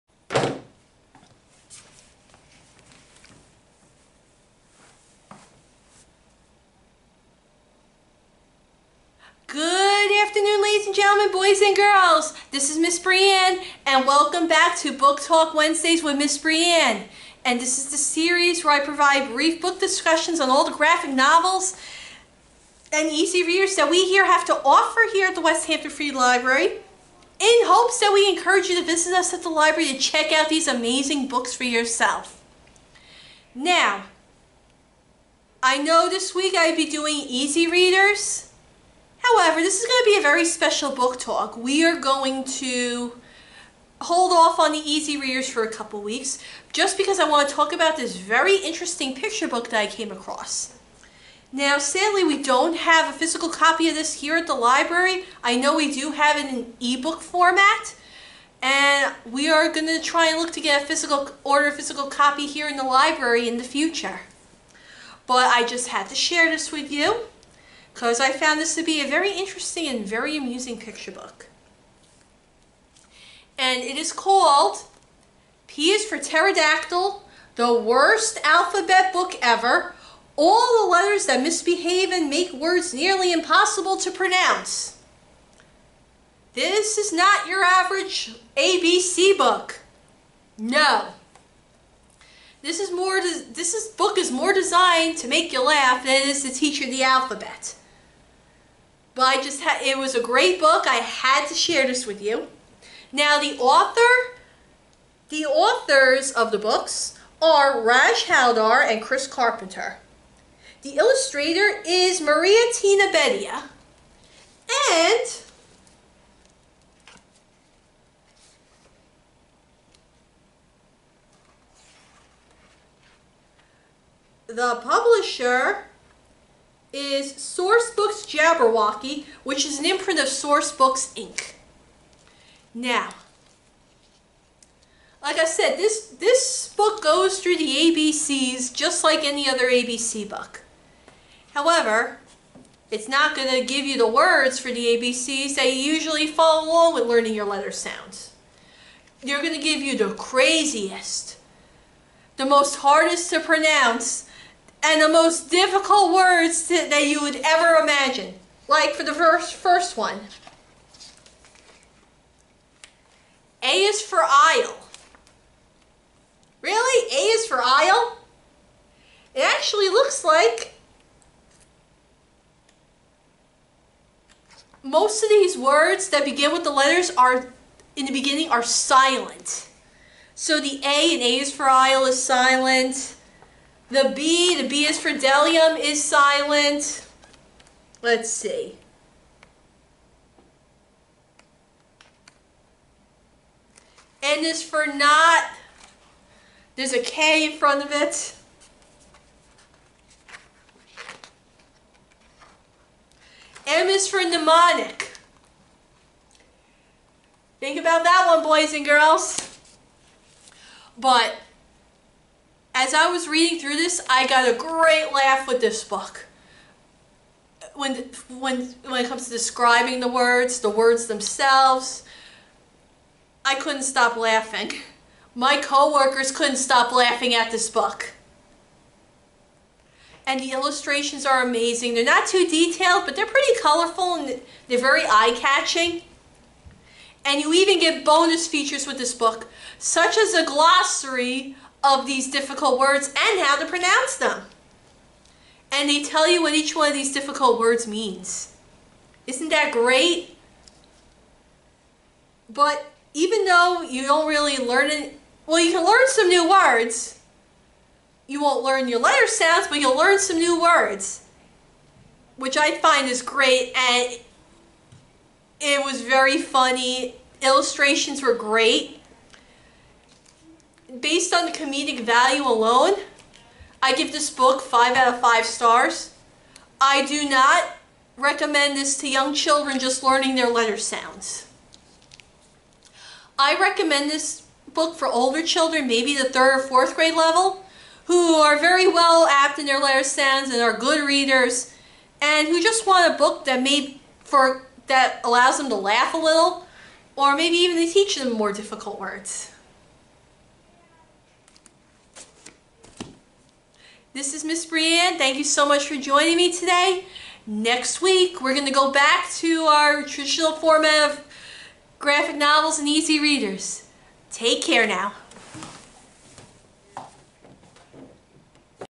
Book Talk Wednesdays with Ms. Brianne - June 9, 2021
Good Afternoon!

My name is Ms. Brianne, and welcome back to "Book Talk Wednesdays with Ms. Brianne!," the series where I provide brief book talks on graphic novels and easy readers.

For this week, I am going to steer away from the easy readers in order to talk about an interesting picture books I came across.

Enhance your learning of the ABCs in the amusing and interesting picture book, "P is for Pterodactyl."  The authors of the book are Raj Haldar and Chris Carpenter, the illustrator is Maria Tina Beddia, and the publisher is Sourcebooks Jabberwocky, an imprint of Sourcebooks, Inc.

"P is for Pterodactyl" is not your run-of-the-mill ABCs picture book.  It is an amazing and hilarious picture book in which young children can learn brand new words that do not sound, at all, like the letters they use.  Children will either words that begin with a silent letter, or words that begin with letters that sound like the letter being presented.  It is a very funny way for your older child to increase their vocabulary.

Bonus features include a glossary of the words presented in the books, as well as a guide on how to pronounce them.

We, sadly, do not have a copy of this book here at the library.  We hope to acquire it in the near future.

If you are interested in any of the books that were talked during these presentations, please visit us at the Idea Place!